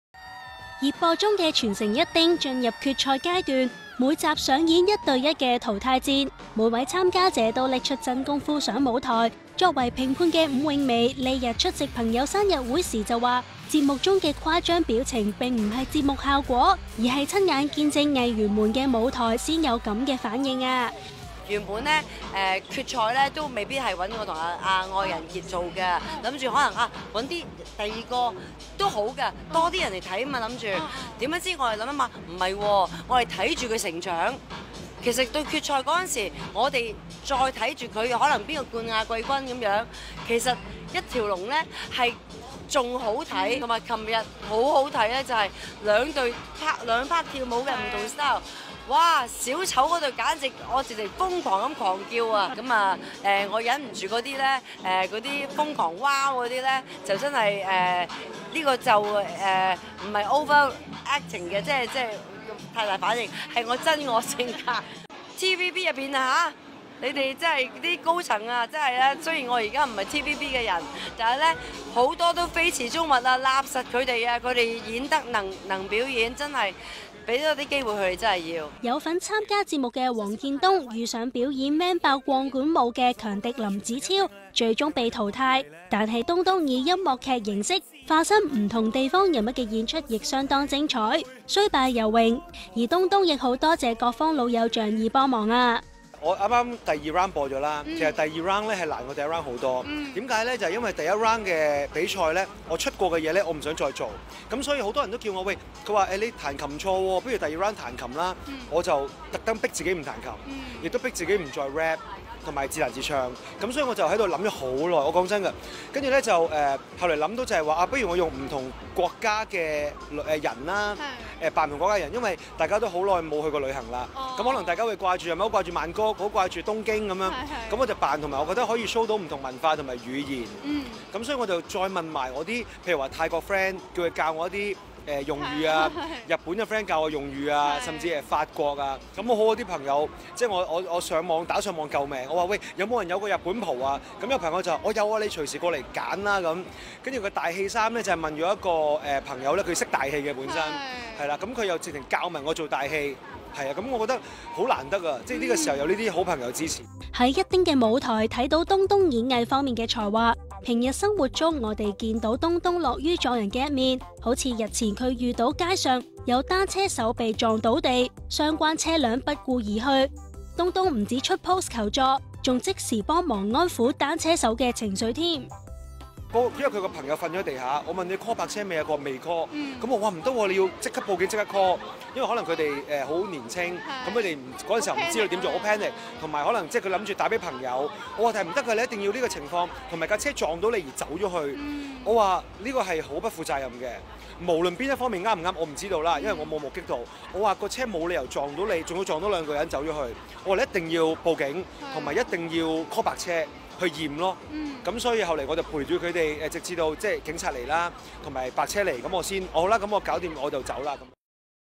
伍詠薇全城一叮誇張表情出自真心｜黃建東為交通意外傷者尋車cam｜HK E News 娛樂新聞報道
才藝表演節目《全城一叮》現正熱播中，每集藝員同事嘅表演都令到評判 伍詠薇 Christine Ng  同主持 Crystal Fung 馮盈盈  喪失表情管理能力，個個睇到嘩嘩聲。呢晚伍詠薇出席朋友嘅生日party，就話自己喺節目入面嘅表情同反應，絕非戲劇效果，全部都係真實反應嚟，伍詠薇仲話好多公司同事都非池中物，呼籲一眾監製要畀多啲機會大家呀！ 而喺節目入面落敗咗畀 AndyLin林子超  嘅 黃建東 Derek Wong  ，就話自己係雖敗猶榮，佢仲話因為想挑戰一下自己，所以先唔演出自己擅長嘅彈鋼琴同Rap，而係挑戰扮唔同國家嘅人唱歌。向來熱心助人嘅東東，日前喺社交平台出post搵車cam，原來嗰晚東東遇到幾位單車手畀架車hit and run，當中兩位單車手更加係受傷倒地，所以東東就幫佢哋上網呼籲，睇下有冇網民見到事發經過，又喺現場陪佢哋等到白車到場先離開。